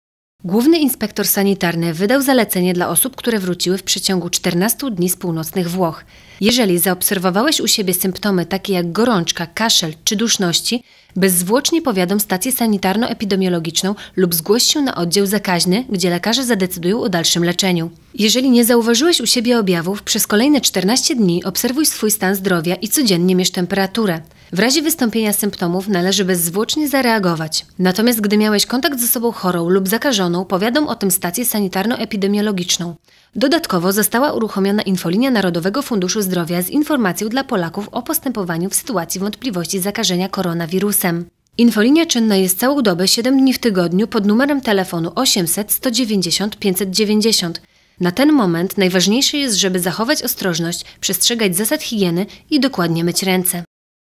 Zalecenia dotyczące koronawirusa
Główny Inspektor Sanitarny wydał zalecenia dla osób, które  wróciły z Włoch, a NFZ otworzył całodobową infolinię z informacjami o wirusie.